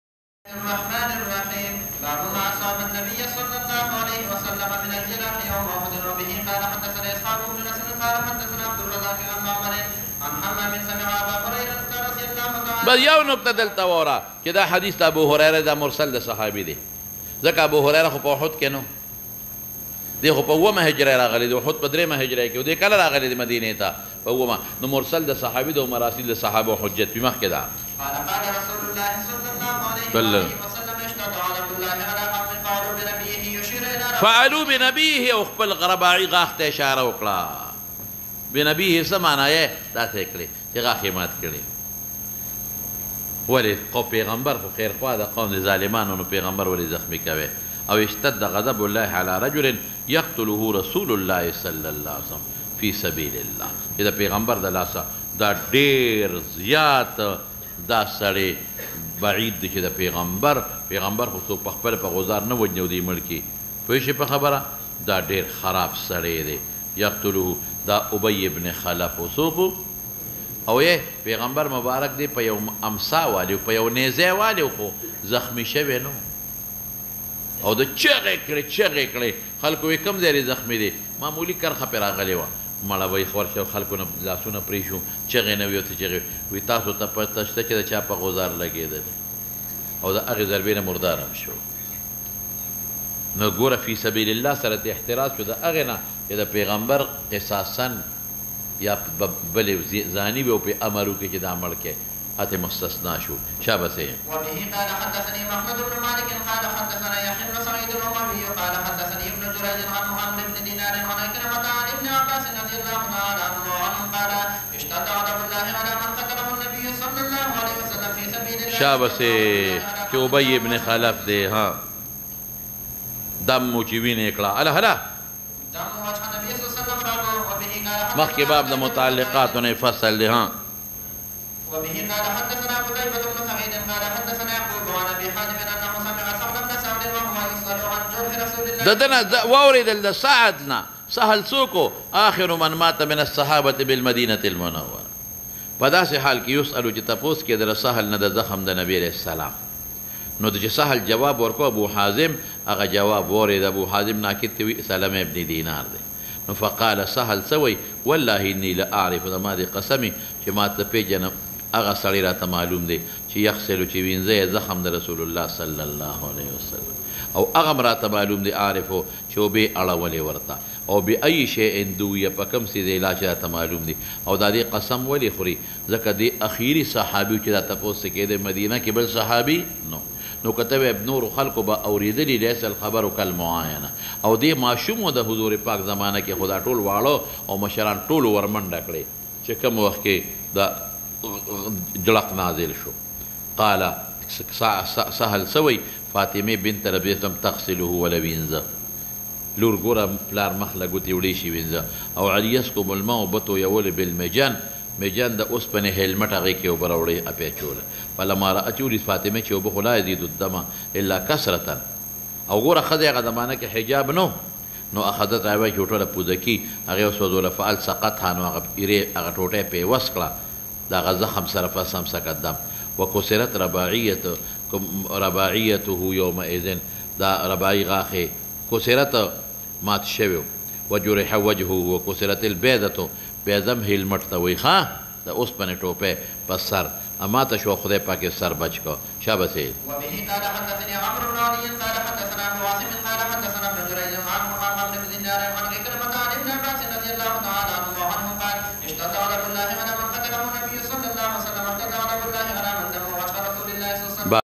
باب ما اصاب النبی صلی اللہ علیہ وسلم من الجراح یوم آخر( بخاری شریف جلد ثانی 2022۔8۔7) شیخ ادریس صاحب
# بخاری شریف
# باب ما اصاب النبی صلی اللہ علیہ وسلم
# درس 2022۔8۔7